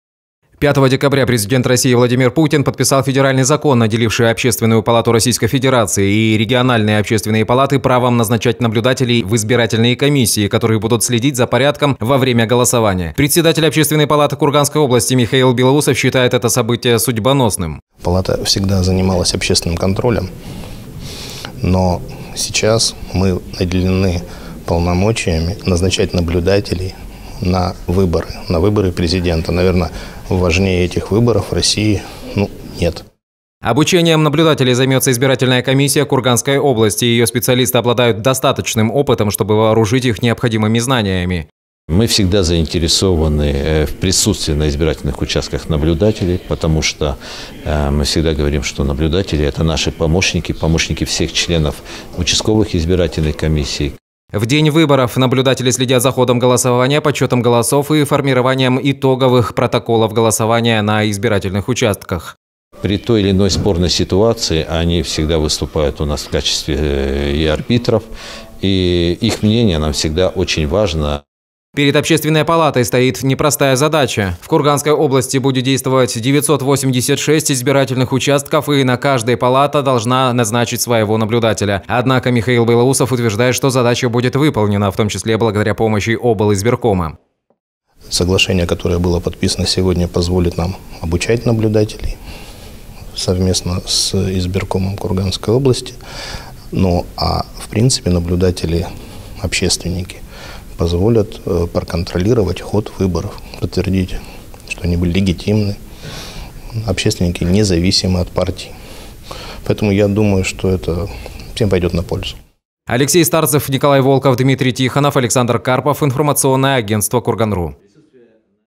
Курганские общественники проследят за чистотой выборов Президента России
Избирком обучит наблюдателей от Общественной палаты Курганской области.